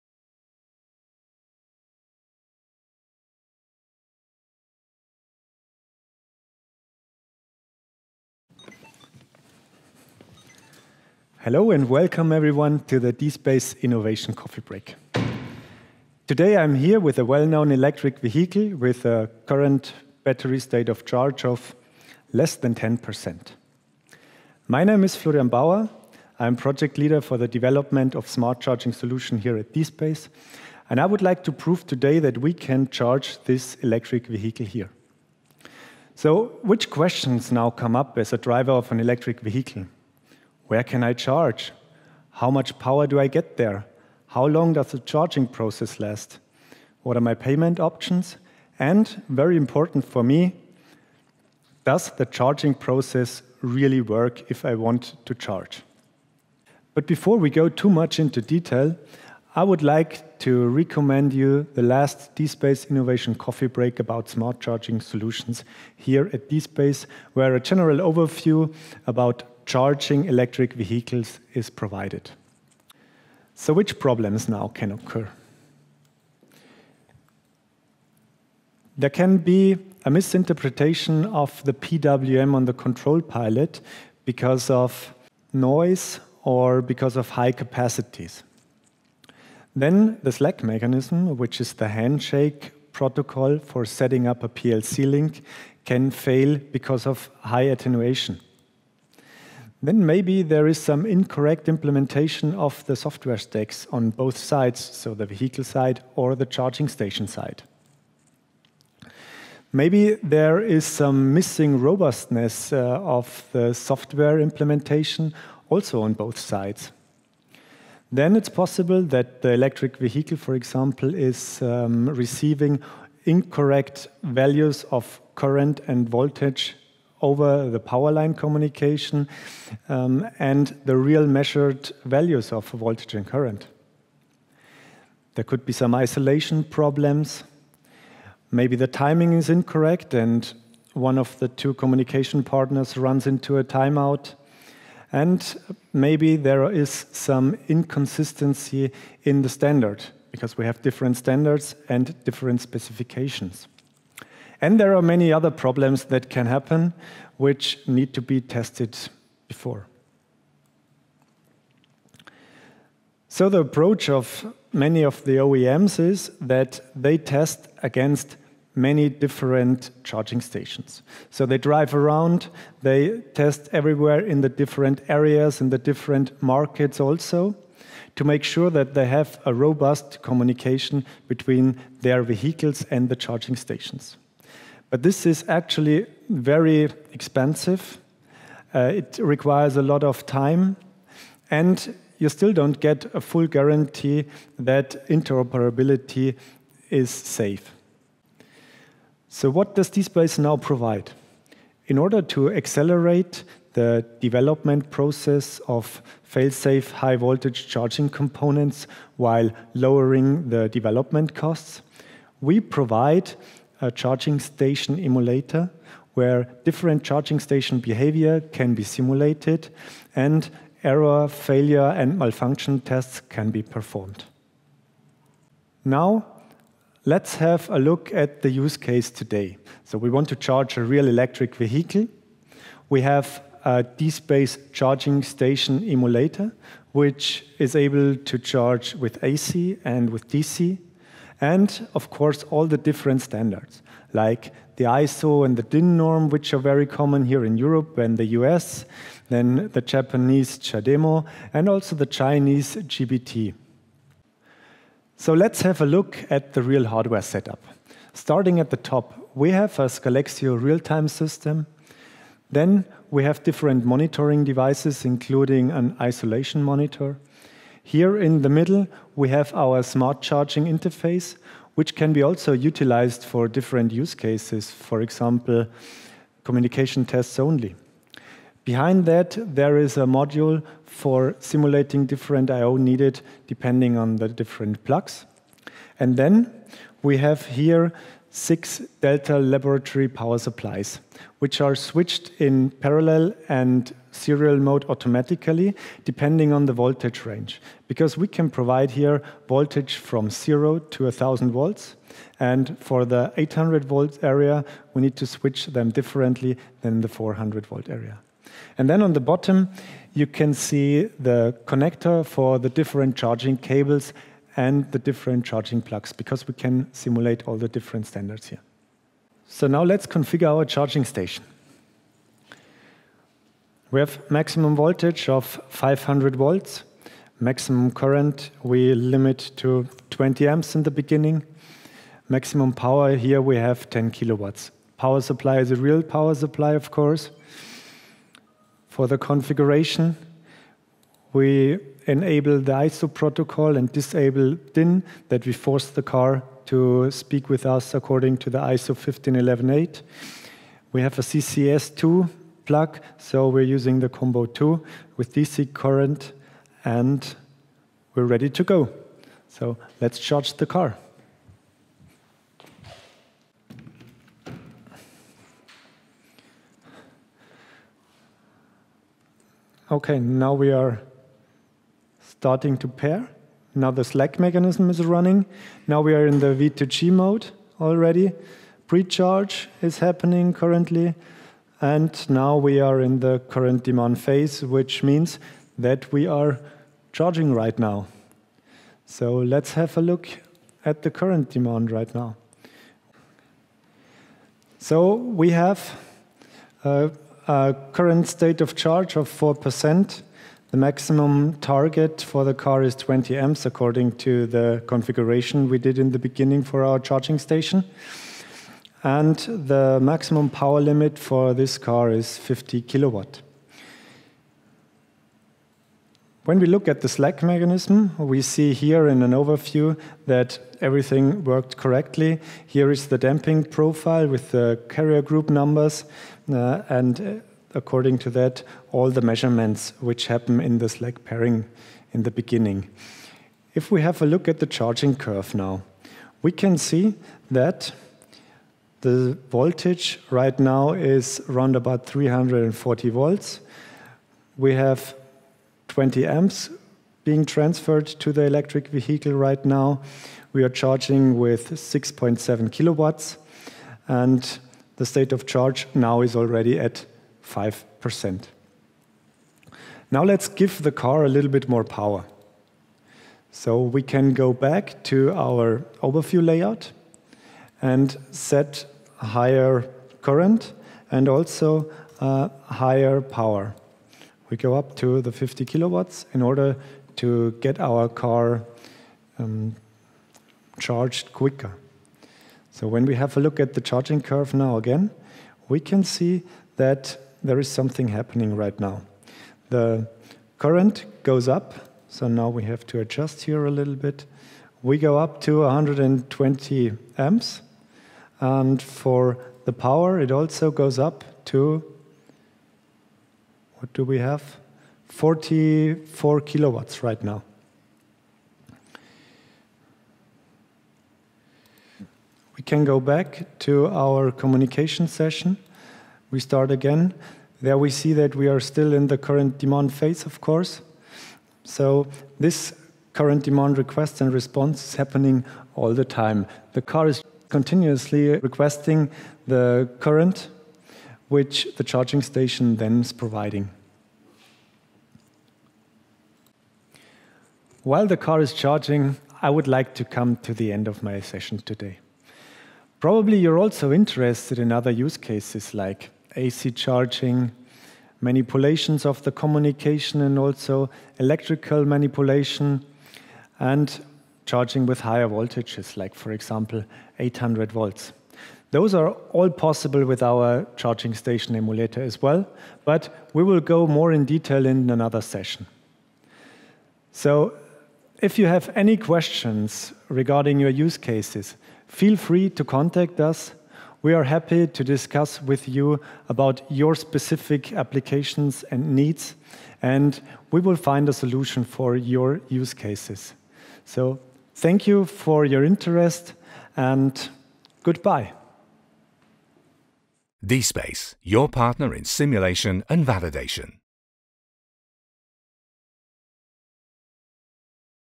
Emulation of Charging Stations with Real Power
The communication between ElectricVehicles and charging infrastructure is a key aspect of reliable ElectricMobility.

To ensure failsafe and robust charging processes, manufacturers need to consider a variety of technological standards, protocols and even possible sources of error. But testing against many different charging stations in different regions of the world drives costs and slows down development.

This is where smart ChargingEmulation comes into play: Watch our Innovation Coffee Break to learn how the dSPACE Smart Charging Solution can accelerate development processes of failsave high-voltage charging components while lowering development costs.